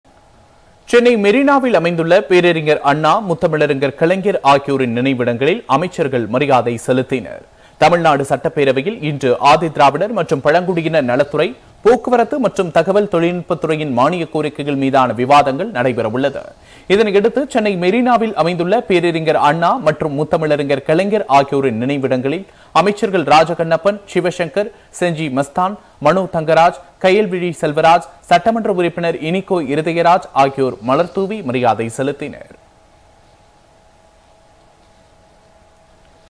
பேரறிஞர் அண்ணா, முத்தமிழ் அறிஞர் கலைஞர் நினைவிடங்களில் அமைச்சர்கள் மரியாதை! | Anna | Kalaignar
பேரறிஞர் அண்ணா, முத்தமிழ் அறிஞர் கலைஞர் நினைவிடங்களில் அமைச்சர்கள் மரியாதை!